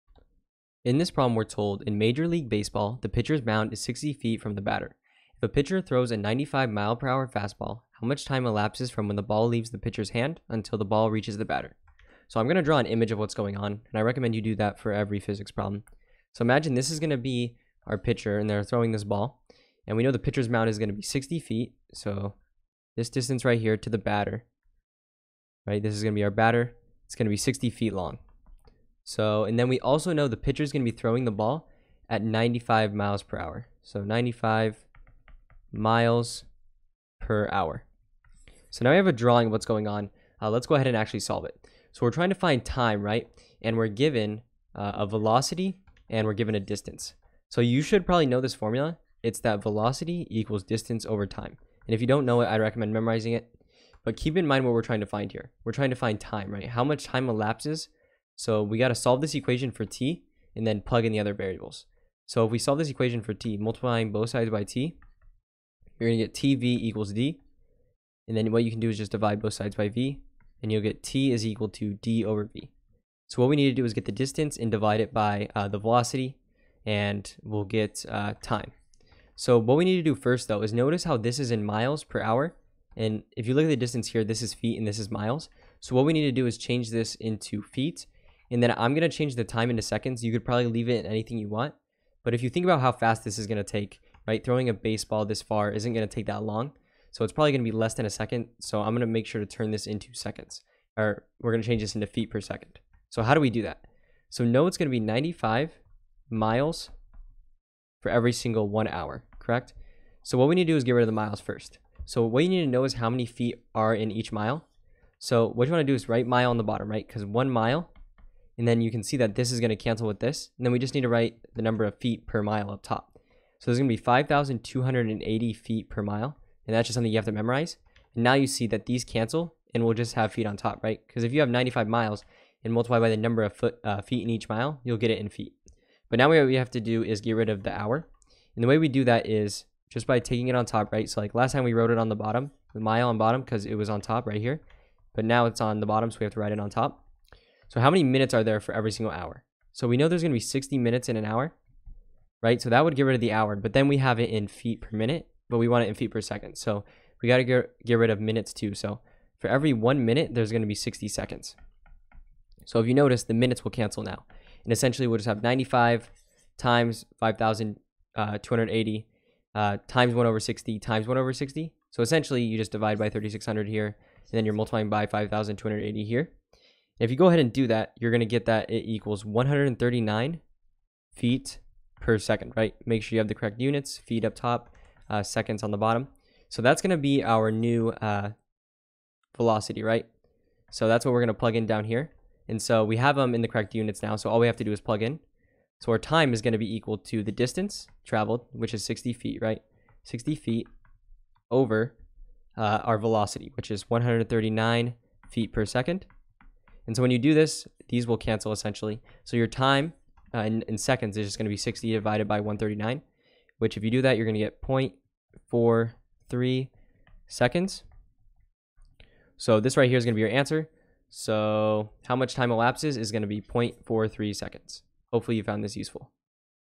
In major league baseball, pitcher’s mound is 60 feet from batter. A pitcher throws a 95 mph fastball
In major league baseball, the pitcher’s mound is 60 feet from
the batter. If a pitcher throws a 95 mph fastball, how much time
elapses from when the ball leaves the pitcher’s hand until the
ball reaches the batter? .